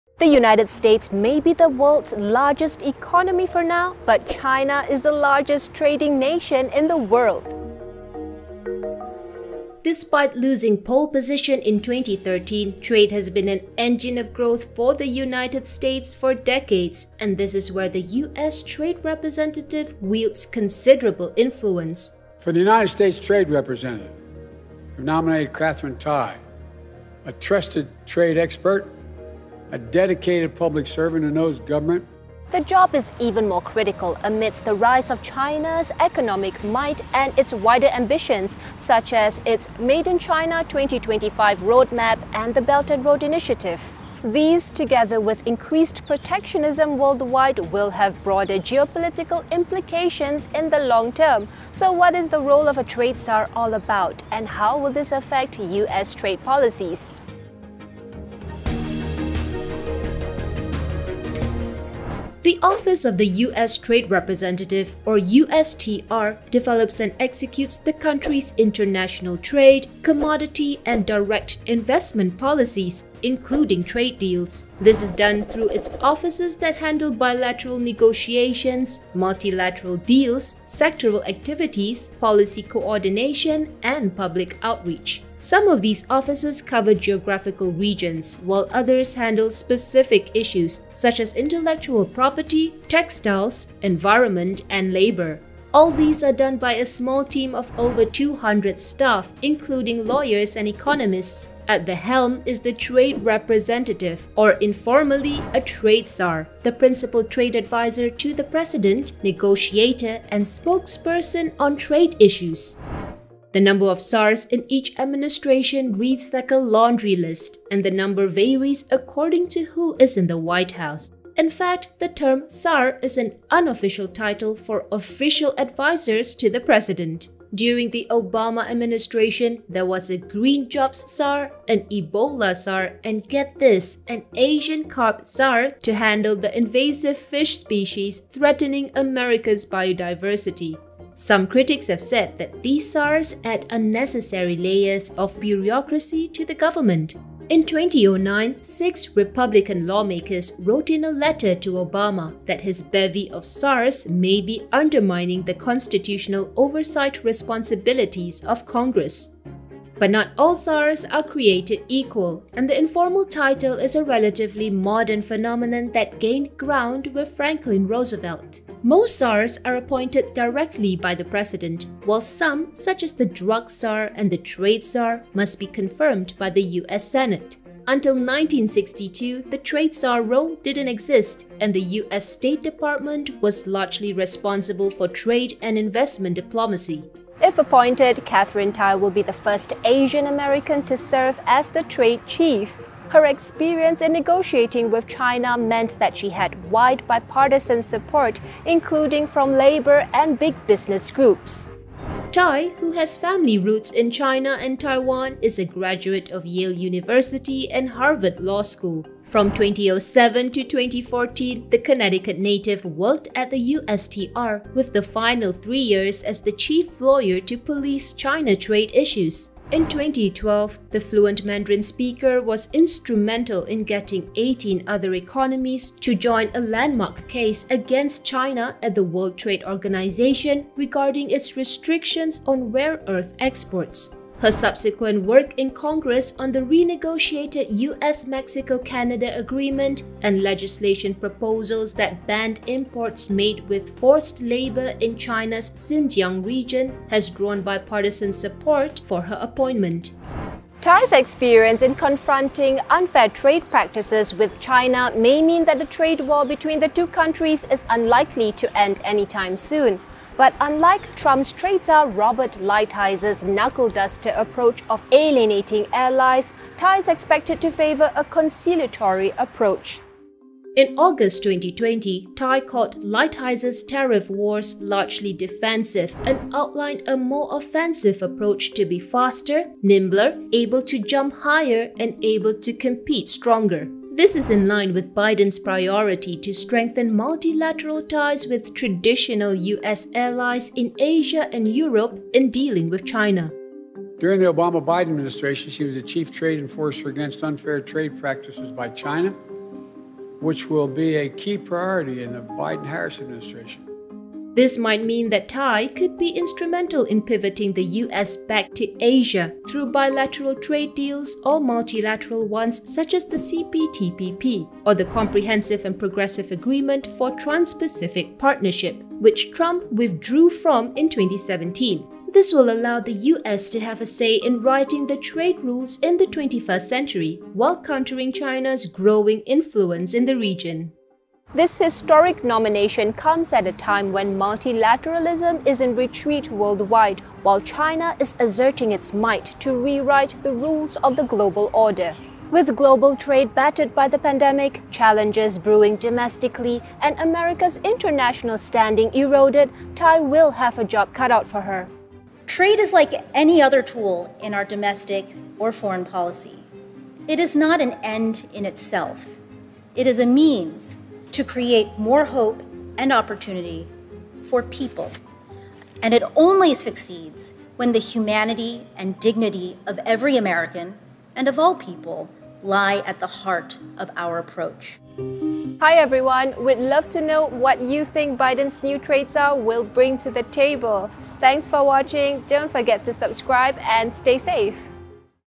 Who is trade czar Katherine Tai? | CNBC Explains
Joe Biden has nominated Katherine Tai as the next U.S. Trade Representative. So, who is she, and what is the role of a trade czar? CNBC’s Nessa Anwar explains.